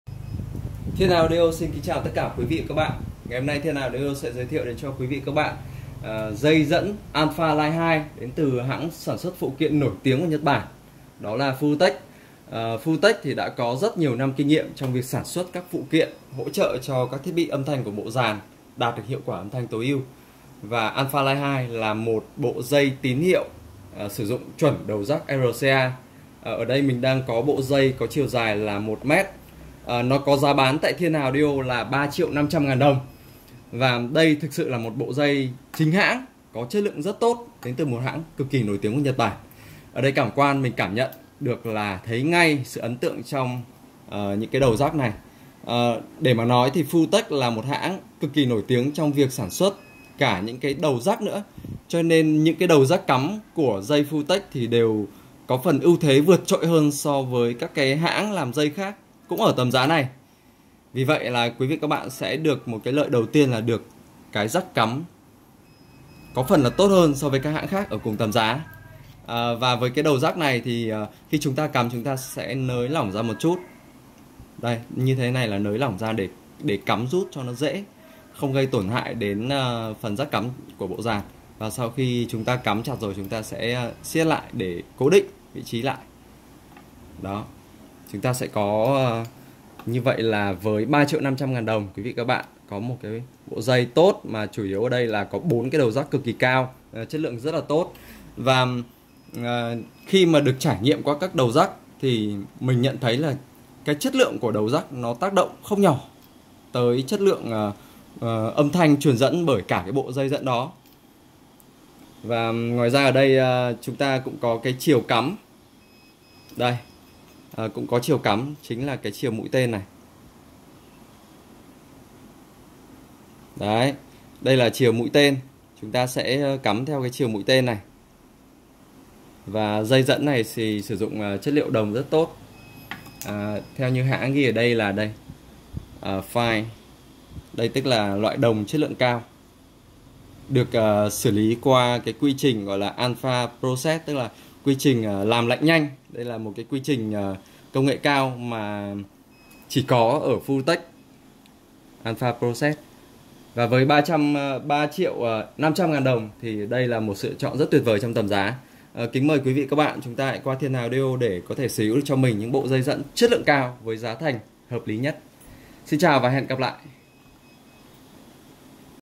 Dây Tín Hiệu Furutech Line 2 - Audio Thiên Hà
Dây Furutech Line 2 được Furutech Ạlpha Design labs làm bằng công nghệ xử lý đông lạnh sâu  âm -280 độ C,  Đầu rắc được thiết kế khoá và cơ khí mạ kim đặc biệt,  Mỗi sản phẩm dù nhỏ bé nhất của Furutech cũng dược làm bằng chất liệu có độ tinh khiết cao  nhất .